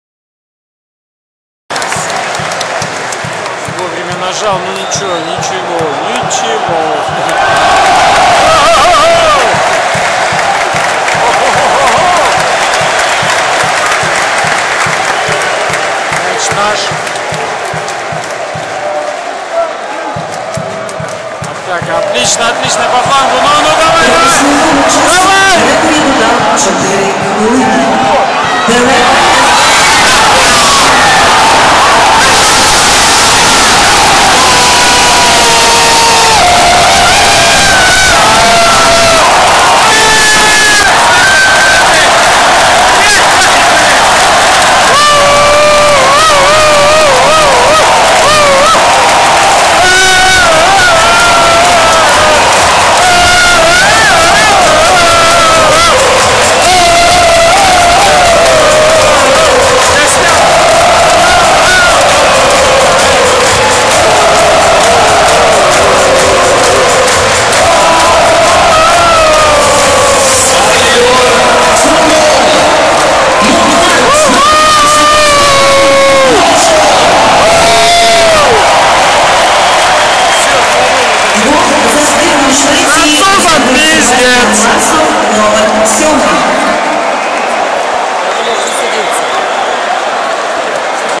Sweden - France 2:0 second goal
Швеция - Франция 2:0 второй гол, фанатское видео